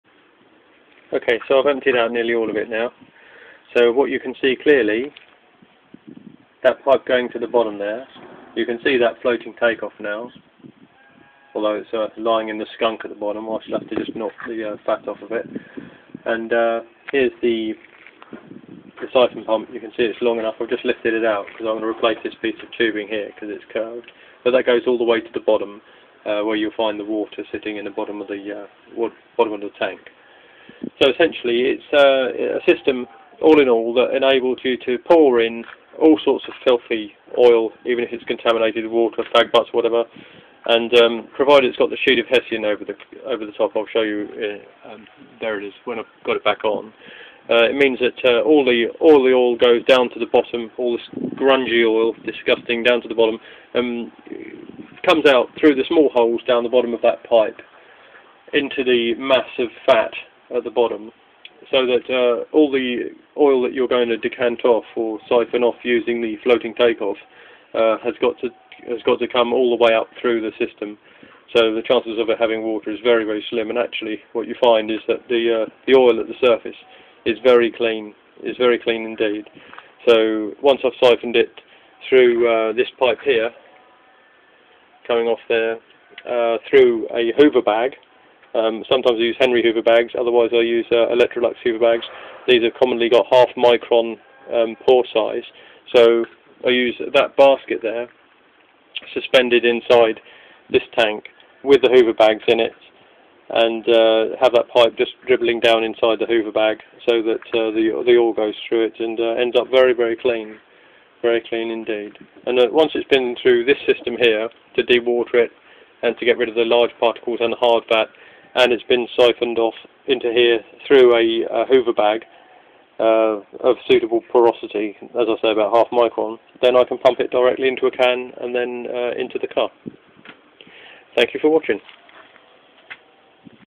Purifying Waste Cooking Oil for Fuel (3/4)
This is a short series showing you my VERY LOW COST, HIGHLY EFFECTIVE system for de-watering, settling and filtering waste oil for use as a diesel supplement/alternative (subject to suitability).
Two Systems are shown; one for absolute  filth contaminated with food/water/cigarette ends/fat, and the other for visibly clear oil only (simpler system).
All components are selected for cheapness and availability. The final filtration is done through vacuum cleaner bags (Henry's are good). they can be burned for fuel when clogged with solid fats (see FATLOG series).

Go ahead and emancipate yourself from fuel slavery.

Other guidance (you must verify as current and valid)
Currently in the UK you do not require a 'Waste Carriers Licence' if you are collecting for your own use; you can use 2,500L/yr before registeing with HMCE to pay duty (about 30p/L), but you must keep a record of what you have used.